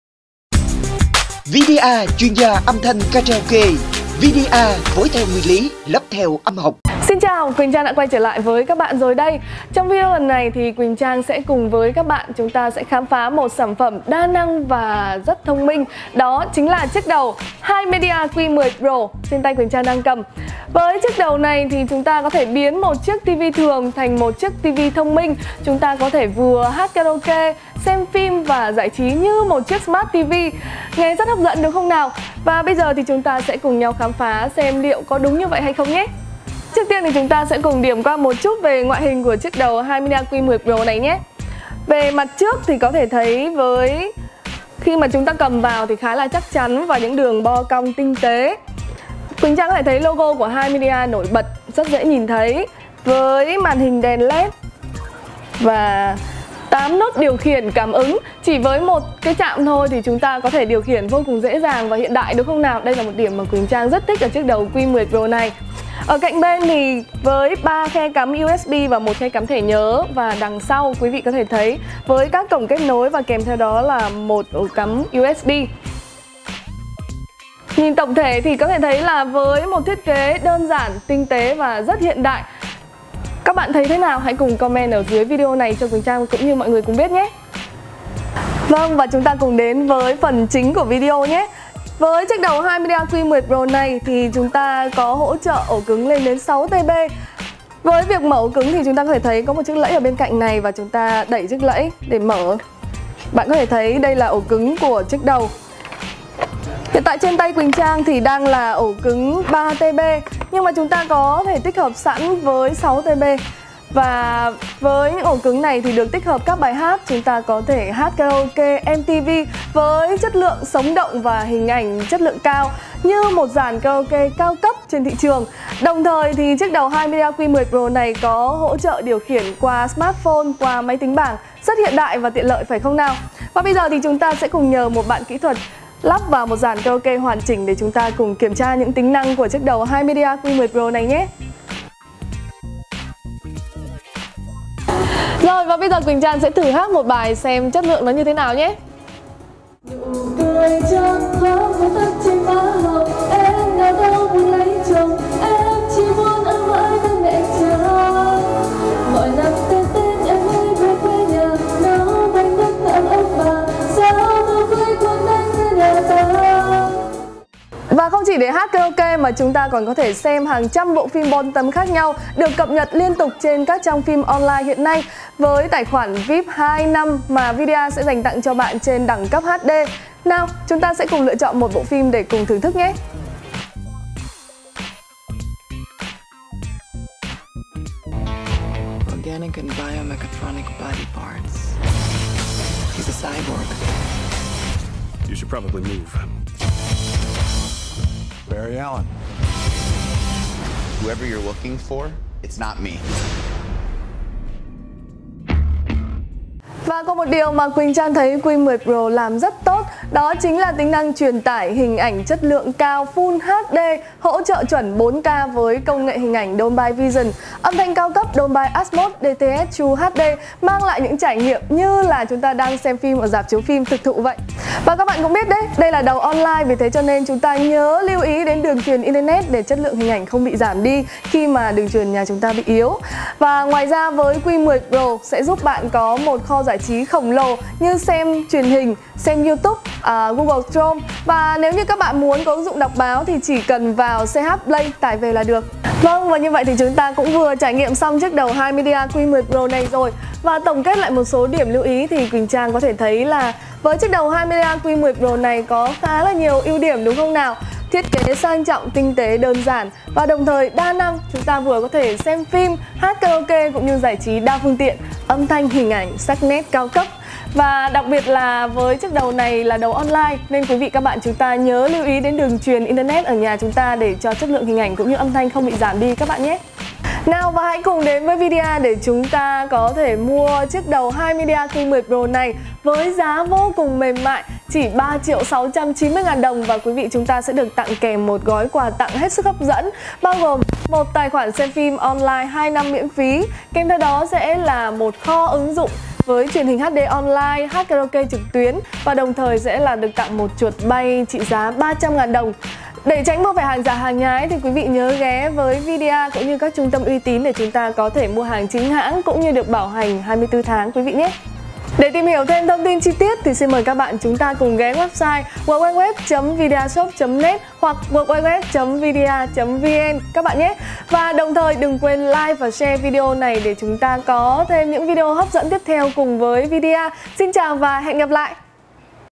Cùng Quỳnh Trang xem phim với đầu phát android box Himedia Q10-Pro đã hơn xem phim rạp
- Mời bạn xem chi tiết sản phẩm tại:
- Đt tư vấn: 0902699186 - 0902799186
-- Nhantin.vidia.vn
207 Tân Phước, P.6, Q.10, TP.HCM